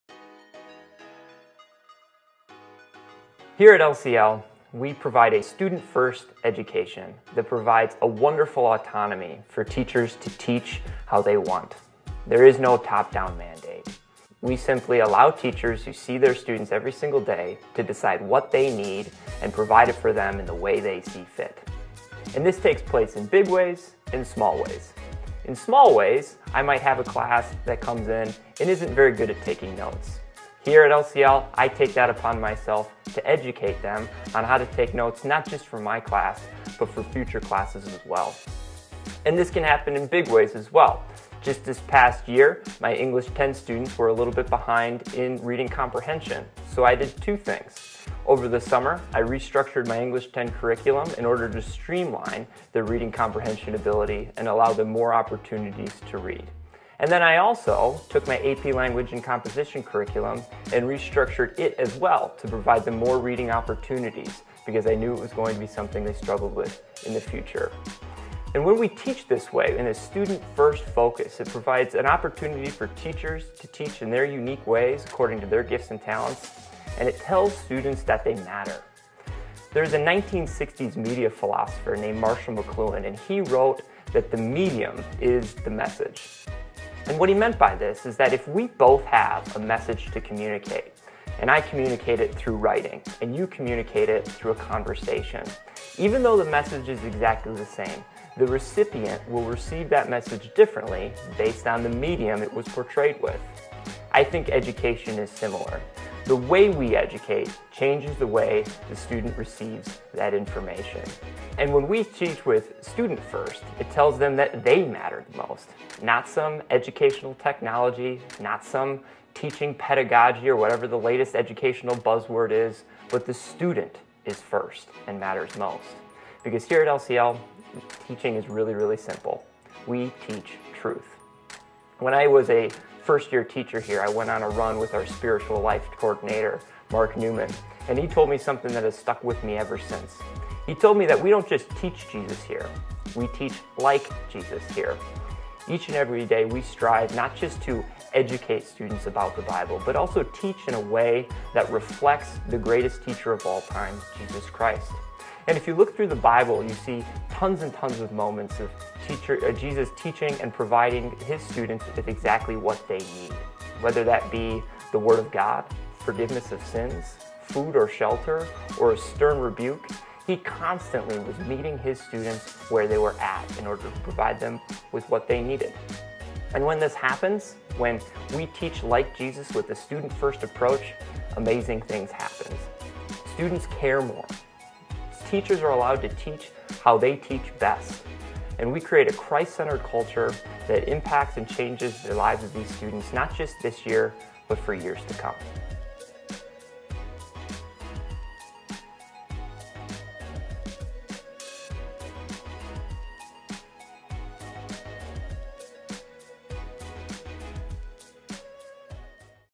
Academics Explained | Mr. Oldenburg
Mr. Oldenburg, English teacher at LCL unpacks the academic structure of LCL and how Jesus is not just what we teach but how we teach.